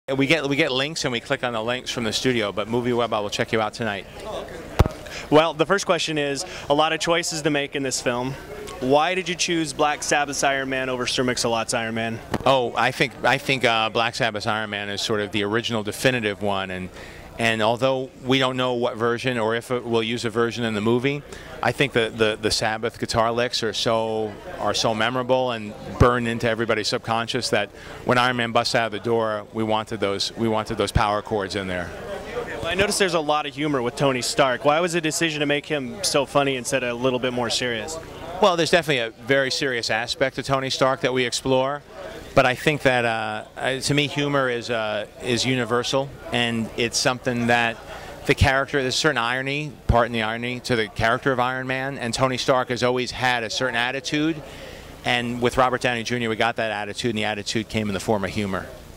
Iron Man - Exclusive: Jon Favreau Interview
MovieWeb goes one-on-one for an exclusive interview with director, Jon Favreau to talk about his latest work with superhero flick Iron Man.